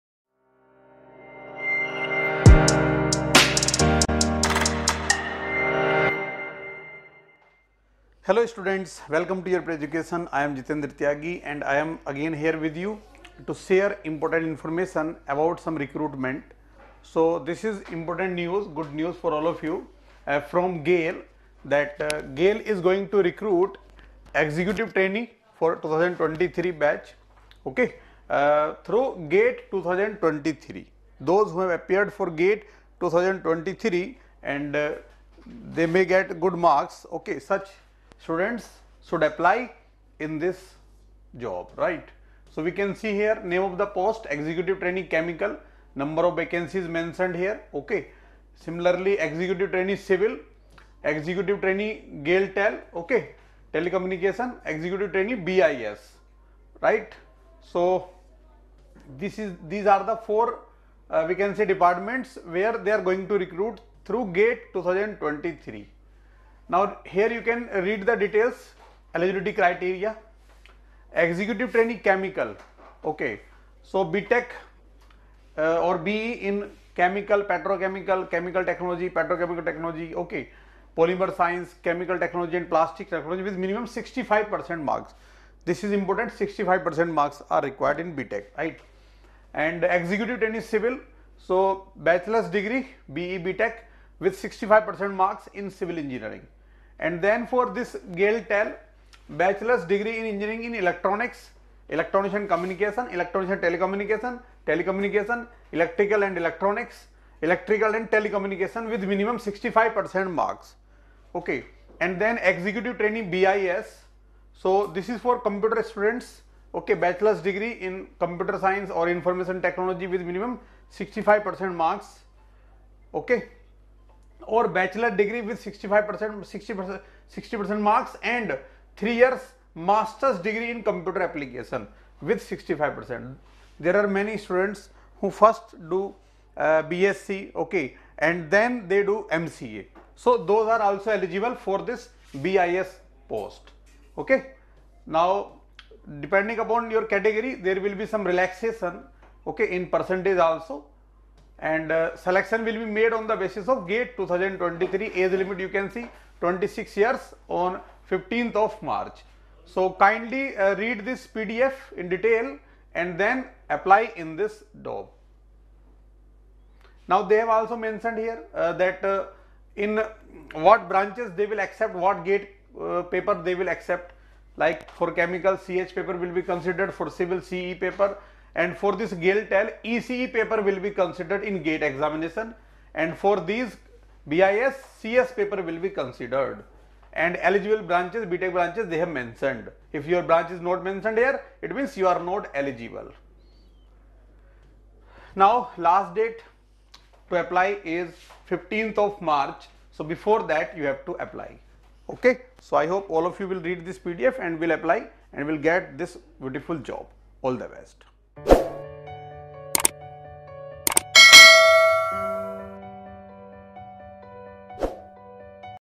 GAIL Recruitment 2023 | EXECUTIVE TRAINEE | GAIL Recruitment through GATE 2023 | Interview Guidance
GAIL (India) Limited is India’s leading natural gas company with diversified interests across the natural gas value chain of trading, transmission, LPG production & transmission, LNG re-gasification, petrochemicals, city gas, E&P, etc. It owns and operates a network of around 13,340 km of natural gas pipelines spread across the length and breadth of country. It is also working concurrently on execution of multiple pipeline projects to further enhance the spread. GAIL commands ~70% market share in gas transmission and has a Gas trading share of over ~ 50% in India. GAIL and its Subsidiaries / JVs also have a formidable market share in City Gas Distribution . In the Liquefied Natural Gas (LNG) market, GAIL has significantly large portfolio. GAIL is also expanding its presence in renewable energy like Solar, Wind and Bio fuel.